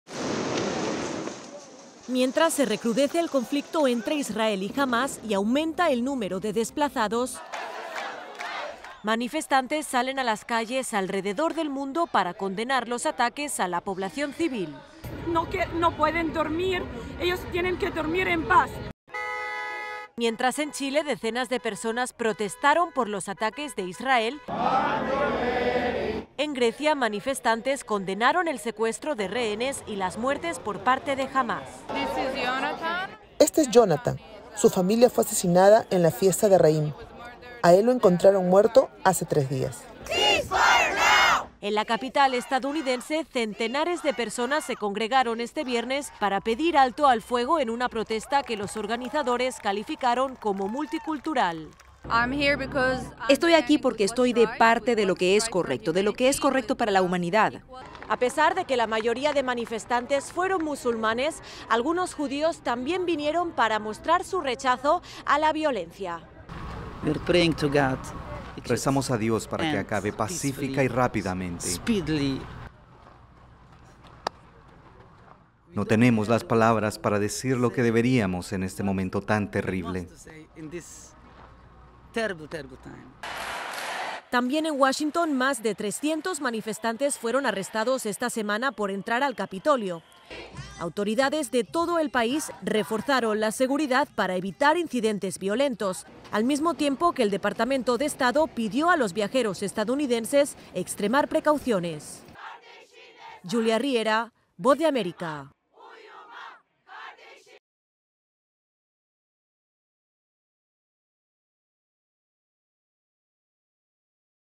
Manifestantes protestan por todo el mundo contra conflicto entre Israel y Hamás
En la última semana, personas de todo el mundo han salido a las calles para condenar la violencia que se vive en Medio Oriente. Desde Washington, informa Júlia Riera de la Voz de América.